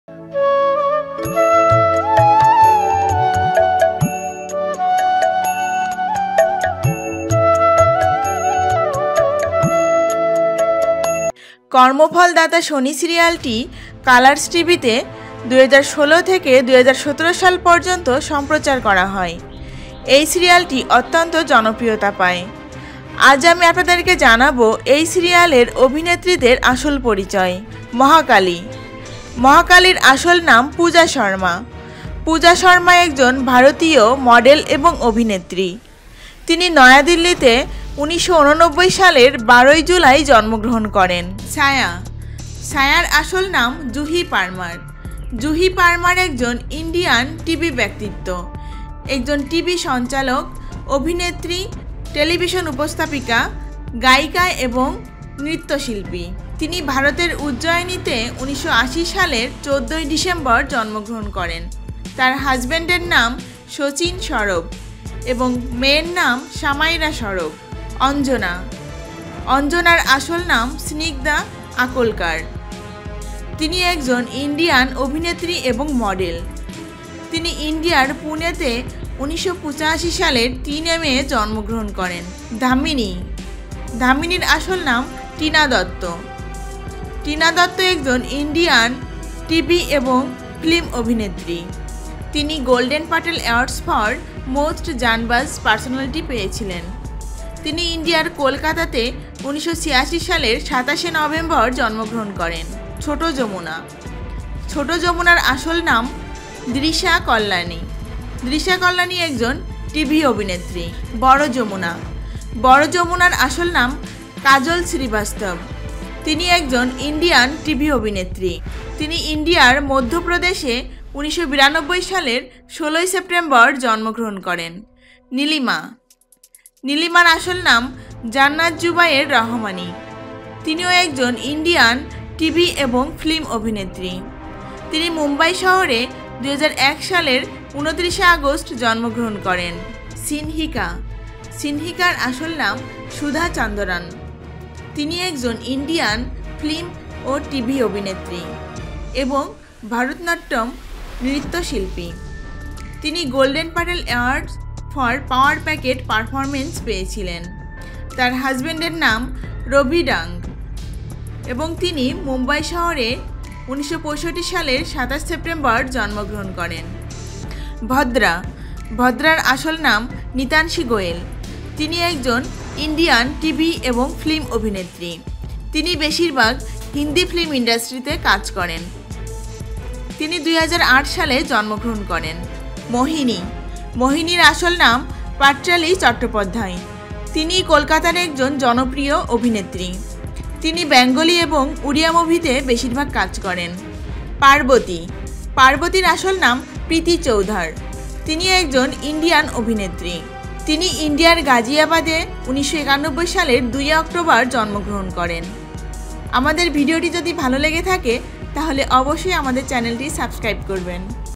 শনি সিরিয়ালের অভিনেত্রীদের আসল পরিচয় । Shani serial actress real identity
1. Goddess Mahakali
Real name-Pooja Sharma
Job-Pooja Sharma is an Indian model and actress
Birthday-July 12, 1989 (age 30 years), New Delhi, India

Real name-Veebha Anand
Job-Veebha Anand is an Indian actress
Birthday-September 8, 1990 (age 29 years), Mumbai, India

2. Sandhya/Chhaya
Real name- Juhi Parmar
Job-Juhi Parmar is an Indian TV personality and an anchor, actress, television presenter, singer and dancer.
Birthday-December 14, 1980 (age 39 years), Ujjain, India
H.B- Sachin Shroff
Children: Samaira Shroff

3. Anjani
Real name-Snigdha Akolkar
Job- Snigdha Akolkar is an Indian actress and model
Birthday- May 3, 1985 (age 34 years), Pune, India

4. Dhamini
Real name- Tina Dutta
Job- Tina Datta, also spelled Tinaa Dattaa, is an Indian film and television actress.
Awards:- Golden Petal Awards for Most Janbaaz Personality
Birthday- November 27, 1986 (age 33 years), Kolkata, India

5. Small Yamuna
Real name- Drisha Kalyani
Job- Drisha Kalyani, Actress: The Sholay Girl. Drisha Kalyani is an actress

Big Yamuna
Real name- Kajol Srivastava
Job- Kajol Srivastava is an Indian actress.
Birthday- . She was born on 16 September 1992 Age 25 Years in Panna, Madhya Pradesh, India

6. Neelima
Real name- Jannat Zubair Rahmani
Job- Jannat Zubair Rahmani is an Indian film and television actress.
Birthday- August 29, 2001 (age 18 years), Mumbai, India

7. Sinhika
Real name- Sudha Chandran
Job- Sudha Chandran is an Indian film and television actress and an accomplished Bharatanatyam dancer.
Awards:- Golden Petal Award for Power Packed Performance, MORE
H.B -Ravi Dang
Birthday- September 27, 1965 (age 54 years), Mumbai, India

8. Bhadra
Real name- Nitanshi Goel
Job- Nitanshi Goel is an Indian Television and film child actress who predominantly works in Hindi film industry
Birthday- 2008 (age 11 years)

9. Mohini
Real name- Patrali Chattopadhyay
Job- Patrali Chattopadhyay is an Indian Actress from Kolkata, West Bengal. Patrali Chattopadhyay is best known of her work in Bengali and Odiya movies.

10. Goddess Parvati
Real name- Preeti Chaudhar
Job- Preeti Chaudhary is a television actress
Birthday- October 2, 1991 (age 28 years), Ghaziabad, India